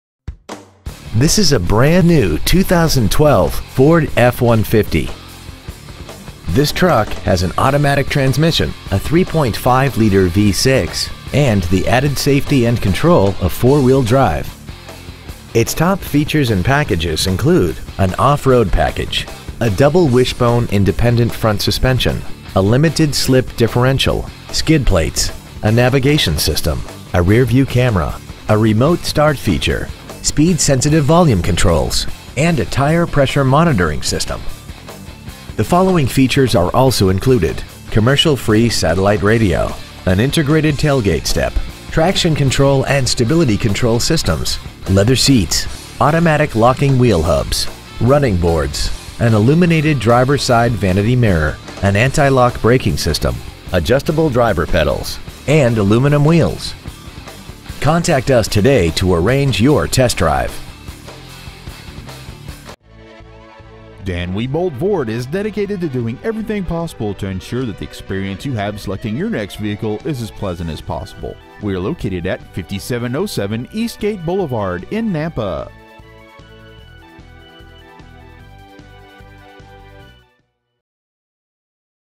2012 FORD F-150 Nampa ID
Year : 2012
 Make : FORD
 Model : F-150
 Engine : 3.5L V6 24V GDI DOHC TWIN TURBO
 Trans . : 6-SPEED AUTOMATIC
 Exterior : OXFORD WHITE
 Miles : 2
 Interior : BLACK
 Stock : 12T2295

 5707 East Gate Blvd.